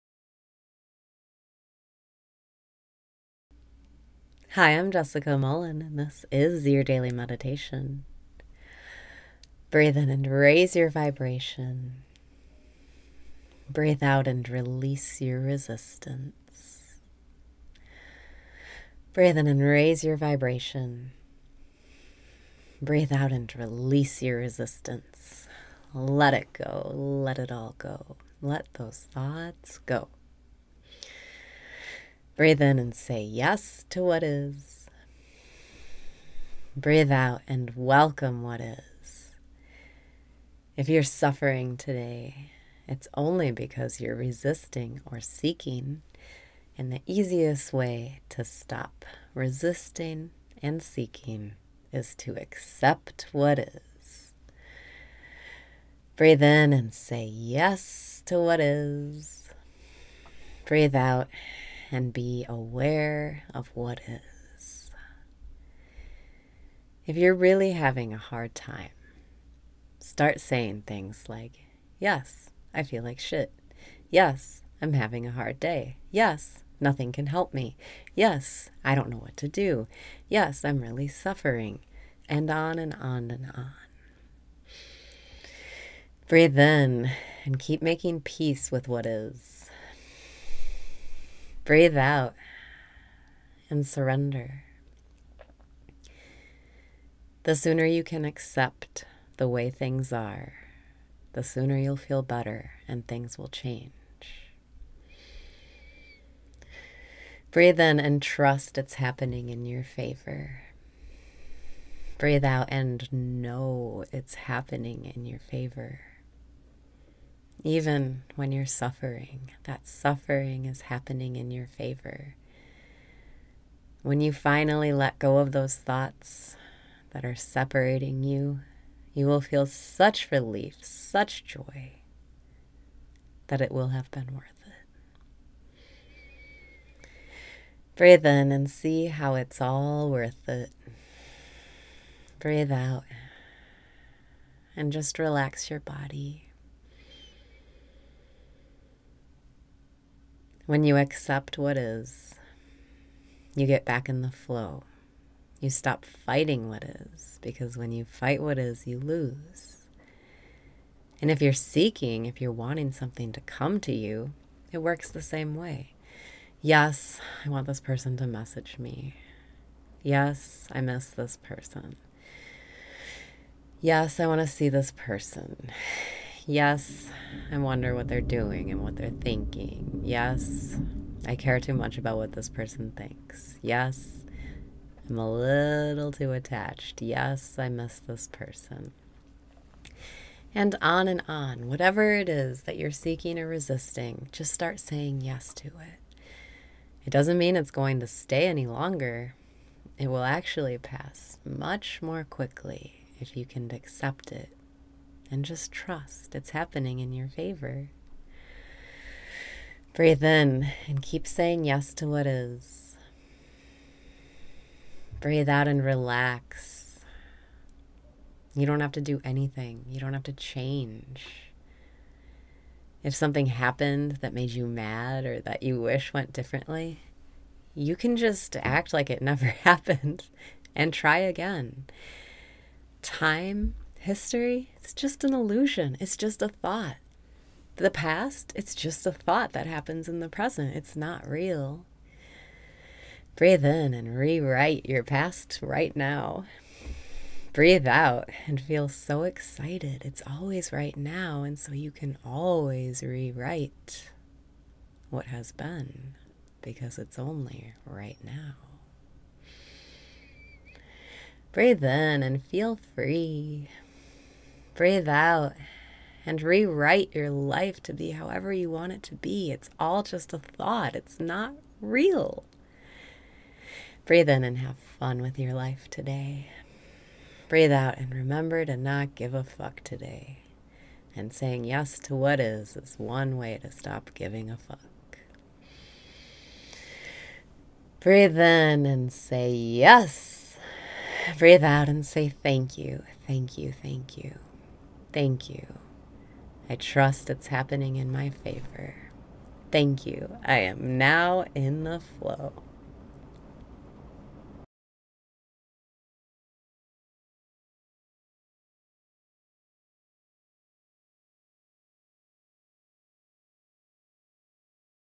Daily Meditation - If You're Having A Hard Time...
+TRANSCRIPT+

Let it go. Let it all go. Let those thoughts go!

If you’re suffering today, it’s only because you’re resisting or seeking. And the easiest way to stop resisting and seeking, is to accept what is.

Breathe out and be aware of what is.

If you’re really having a hard time, start saying things like, “Yes I feel like shit. Yes I’m having a hard day. Yes nothing can help me. Yes I don’t know what to do. Yes I’m really suffering.” And on and on and on…

Breathe in and keep making peace with what is.

The sooner you can accept the way things are, the sooner you’ll feel better and things will change.

Breathe in and trust it’s happening in your favor.
Breathe out and know it’s happening in your favor.

Even when you’re suffering, that suffering is happening in your favor. When you finally let go of those thoughts that are separating you, you will feel such relief, such joy, that it will have been worth it.

Breathe in and see how it’s all worth it.
Breathe out and just relax your body.

When you accept what is, you get back in the flow. You stop fighting what is. Because when you fight what is, you lose. And if you’re seeking—if you’re wanting something to come to you—it works the same way. “Yes I want this person to message me. Yes I miss this person. Yes I want to see this person. Yes I wonder what they’re doing and what they’re thinking. Yes I care too much about what this person thinks. Yes I’m a little too attached. Yes I miss this person.” And on and on.

Whatever it is that you’re seeking or resisting, just start saying yes to it. It doesn’t mean it’s going to stay any longer. It will actually pass much more quickly if you can accept it and just trust it’s happening in your favor!

Breathe in and keep saying yes to what is.
Breathe out and relax. You don’t have to do anything.

You don’t have to change. If something happened that made you mad, or that you wish went differently, you can just act like it never happened and try again. Time, history—it’s just an illusion. It’s just a thought. The past is just a thought that happens in the present. It’s not real.

Breathe in and rewrite your past right now!
Breathe out and feel so excited!

It’s always right now and so you can always rewrite what has been. Because it’s only right now.

Breathe in and feel free!
Breathe out and rewrite your life to be however you want it to be. It’s all just a thought! It’s not real!

Breathe in and have fun with your life today!
Breathe out and remember to not give a fuck today!

And saying yes to what is is one way to stop giving a fuck.

Thank you thank you thank you. Thank you! I trust it’s happening in my favor. Thank you! I am now in the flow!